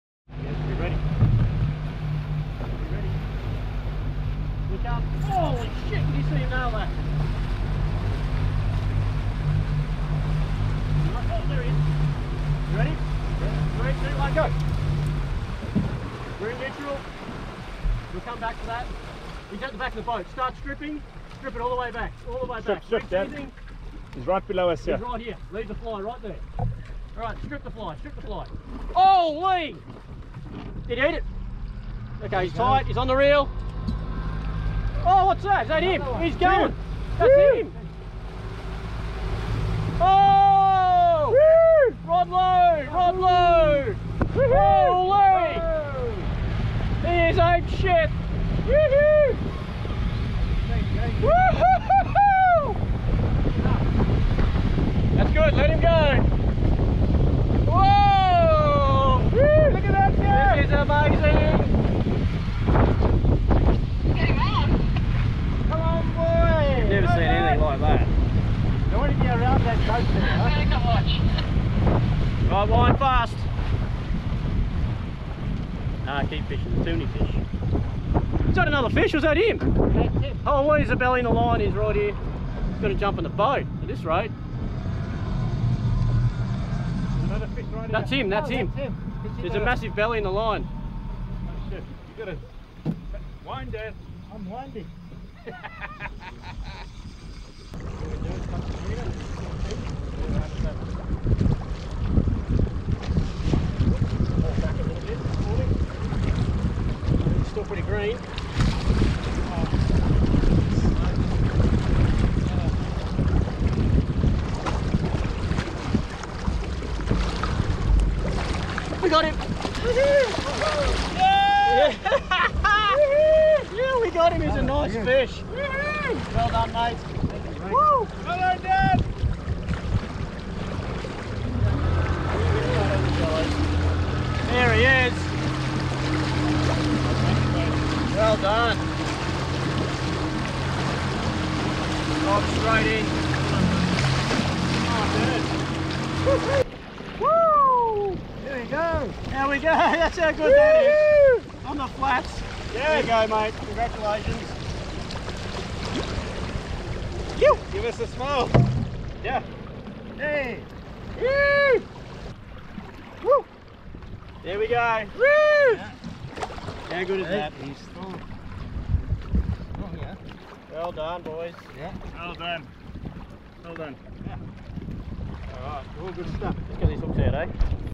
INSANE Black Marlin on Fly Bite! Hervey Bay
Guests targeting juvenile black marlin on fly on the flats of Fraser Island, Hervey Bay Queensland Australia.
Peak season to target these amazing fish is October to December.